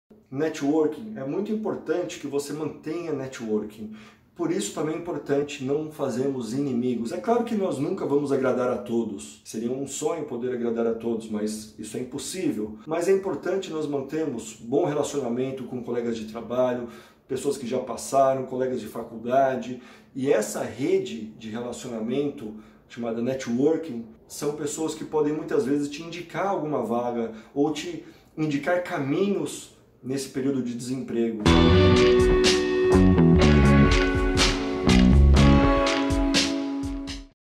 Aumente o seu Networking Consiga Seu Melhor Emprego
Opa! Tudo bem? Aqui é o Laimons =)

Mantenha e aumente o seu networking, essa pode ser a chave para conquistar uma nova oportunidade. Afinal, são pessoas que certamente viram você em ação, por isso poderiam indicá-lo com tranquilidade.

E se você gostou dessas dicas, deixe seus comentários aqui embaixo. Diga pra mim se há algum assunto ou dica que você gostaria de receber. E se esse conteúdo fez sentido pra você compartilha, você pode ajudar aquela pessoa que você nem sabe que está precisando. 😉

Forte abraço!